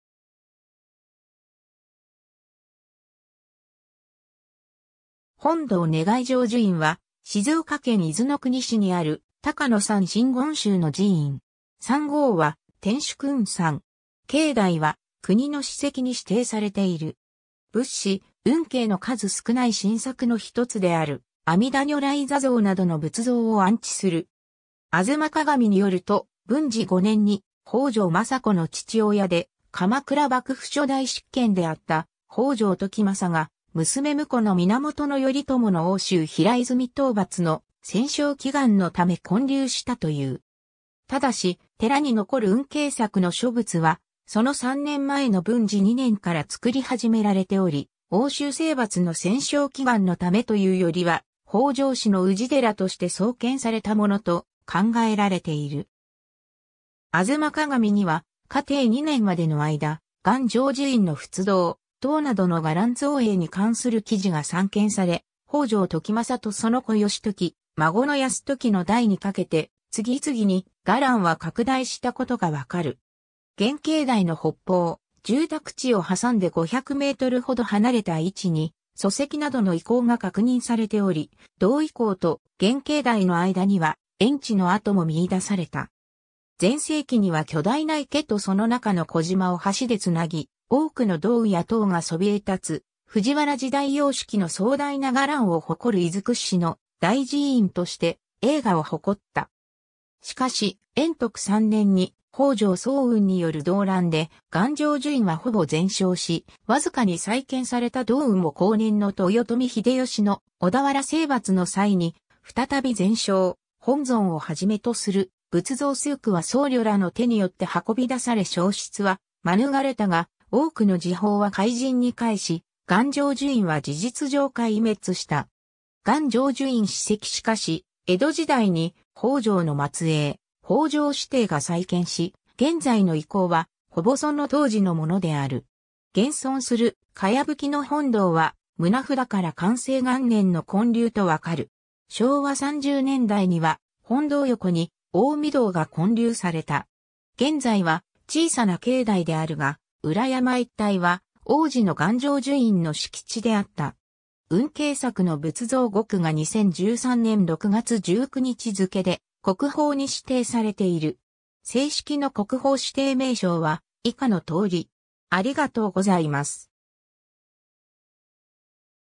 願成就院
日本の国宝_(彫刻)
本堂 願成就院（がんじょうじゅいん）は、静岡県伊豆の国市にある、高野山真言宗の寺院。
山号は天守君山。
境内は国の史跡に指定されている。
仏師運慶の数少ない真作の一つである阿弥陀如来坐像などの仏像を安置する。
 『吾妻鏡』によると、文治5年（1189年）に北条政子の父親で鎌倉幕府初代執権であった北条時政が、娘婿の源頼朝の奥州平泉討伐の戦勝祈願のため建立したという。
ただし、寺に残る運慶作の諸仏はその3年前の文治2年（1186年）から造り始められており、奥州征伐の戦勝祈願のためというよりは、北条氏の氏寺として創建されたものと考えられている。
 『吾妻鏡』には、嘉禎2年（1236年）までの間、願成就院の仏堂、塔などの伽藍造営に関する記事が散見され、北条時政とその子義時、孫の泰時の代にかけて次々に伽藍は拡大したことがわかる。
現境内の北方、住宅地を挟んで500メートルほど離れた位置に礎石などの遺構が確認されており、同遺構と現境内の間には苑池の跡も見出された。
全盛期には巨大な池とその中の小島を橋でつなぎ、多くの堂宇や塔がそびえ立つ、藤原時代様式の壮大な伽藍を誇る伊豆屈指の大寺院として栄華を誇った。
 しかし延徳3年（1491年）に北条早雲による動乱で願成就院はほぼ全焼し、僅かに再建された堂宇も後年の豊臣秀吉の小田原征伐の際に再び全焼、本尊を始めとする仏像数躯は僧侶らの手によって運び出され焼失は免れたが多くの寺宝は灰燼に帰し、願成就院は事実上壊滅した。
 願成就院史跡 しかし江戸時代に北条の末裔、北条氏貞が再建し、現在の遺構はほぼその当時のものである。
現存する茅葺の本堂は、棟札から寛政元年（1789年）の建立とわかる。
 昭和30年代には本堂横に大御堂が建立された。
現在は小さな境内であるが、裏山一帯は往時の願成就院の敷地であった。
 運慶作の仏像5躯（大御堂安置）が2013年（平成25年）6月19日付けで国宝に指定されている（5躯一括指定のため、国宝指定件数としては1件）。
 正式の国宝指定名称は以下のとおり。